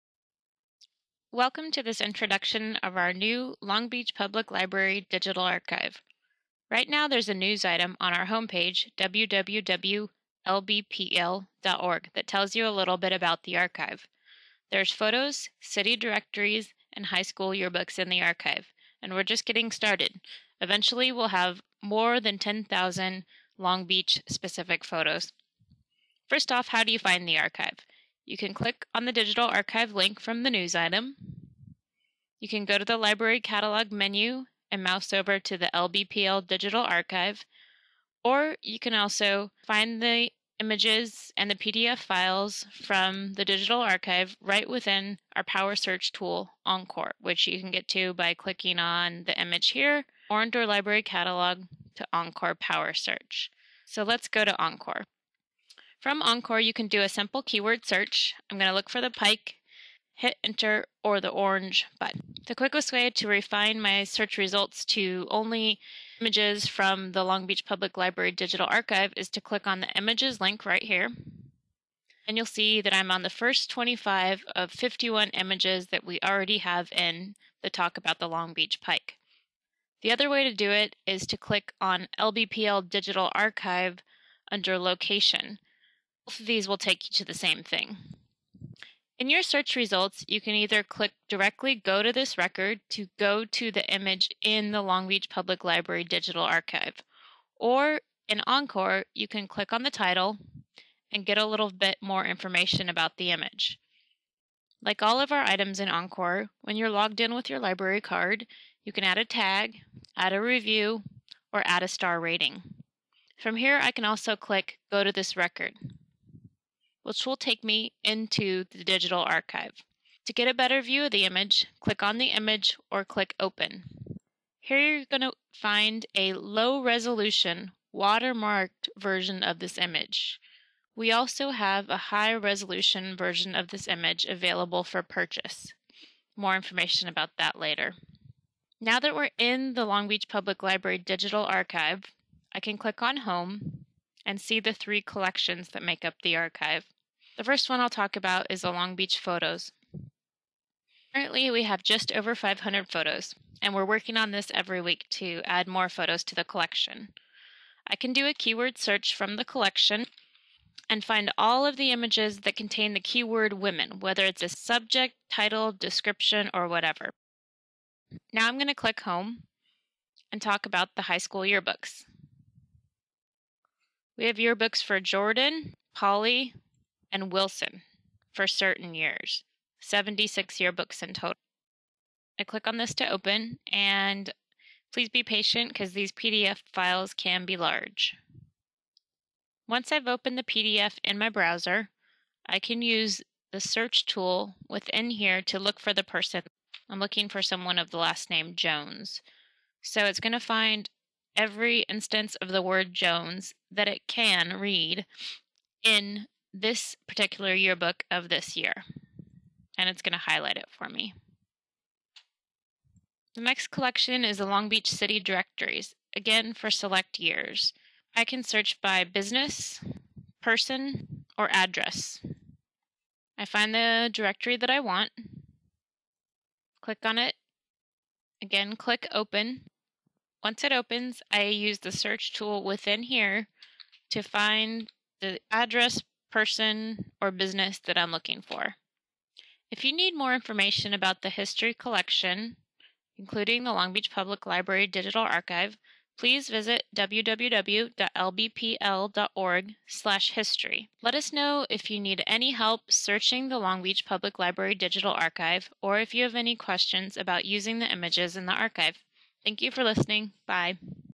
LBPL Digital Archive Intro
A short introduction to our new LBPL Digital Archive. See how to search Long Beach photos, Long Beach high school yearbooks and Long Beach city directories. Explore the collections from the LBPL Digital Archive home or within our library power search, Encore. Ask a librarian for help!